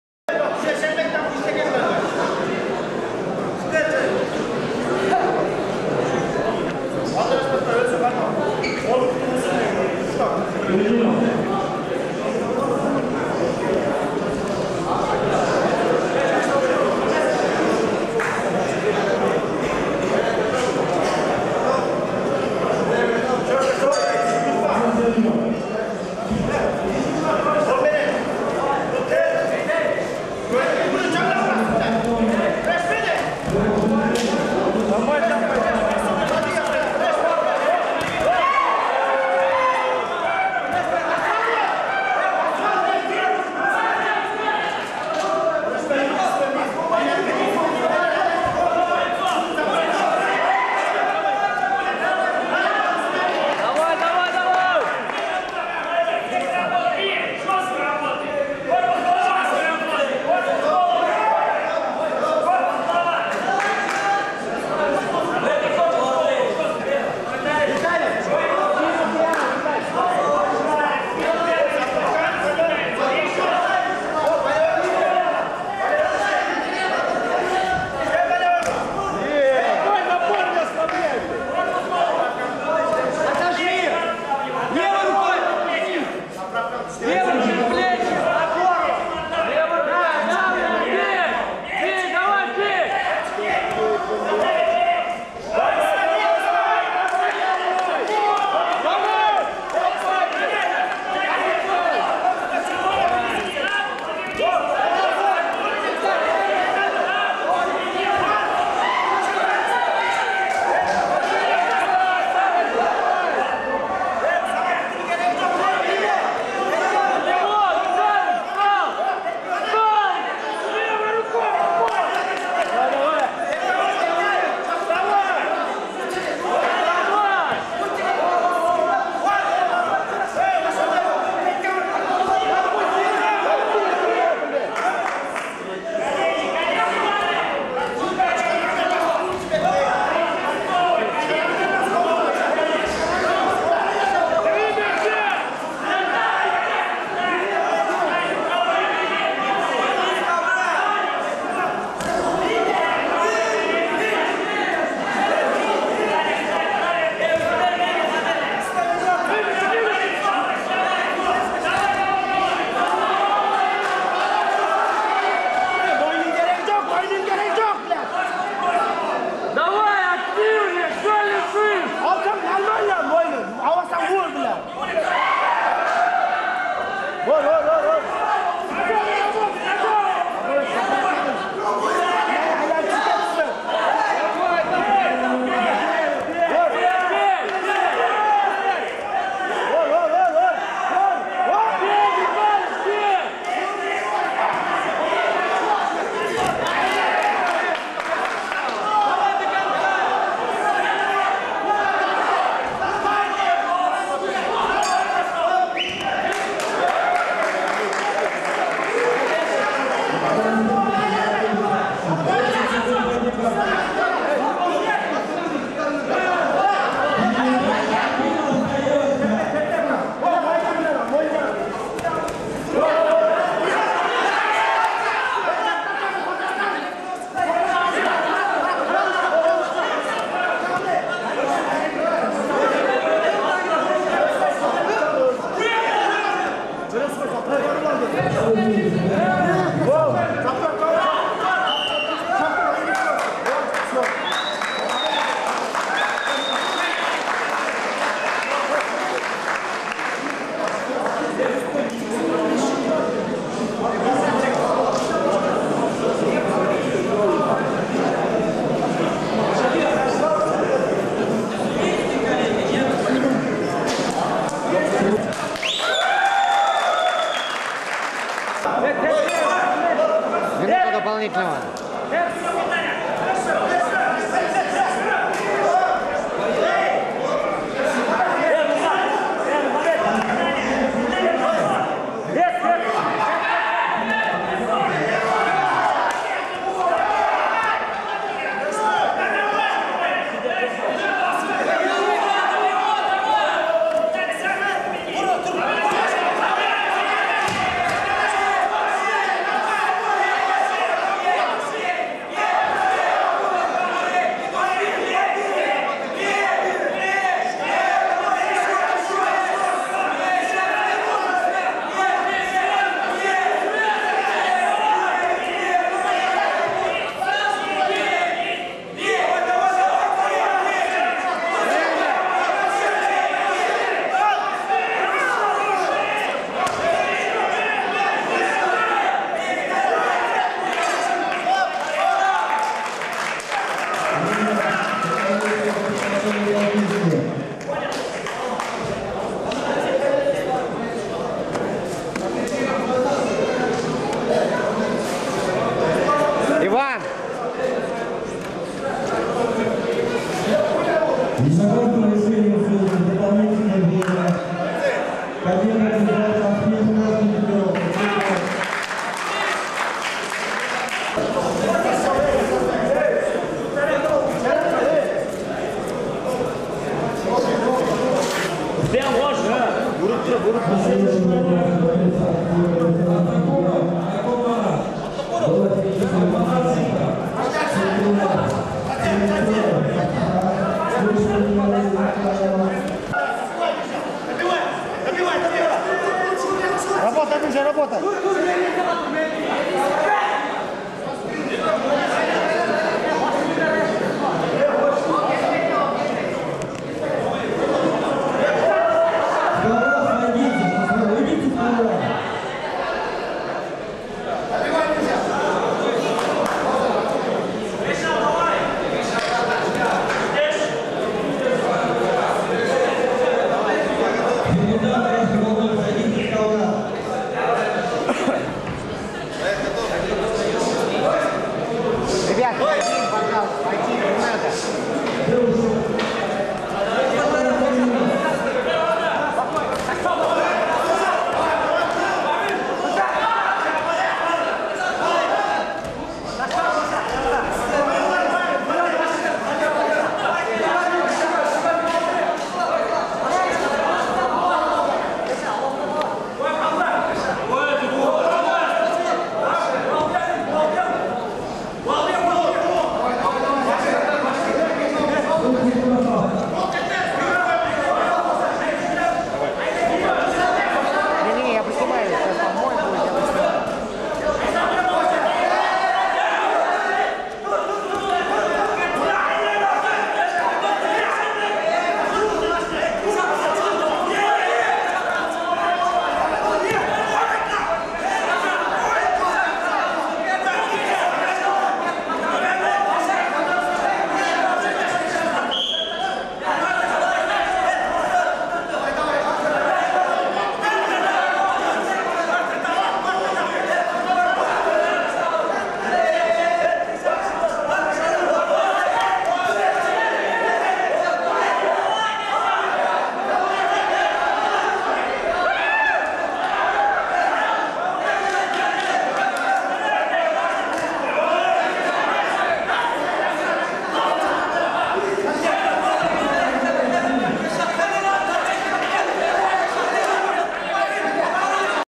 Турнир по панкратиону "Наследие Спарты" 6часть
Архив.06.10.2012г Турнир для спортсменов до 1 разряда включительно.
Турнир по панкратиону "Наследие Спарты" организован и проведен Федерацией панкратиона города Москвы совместно с МЦЕ "Спарта".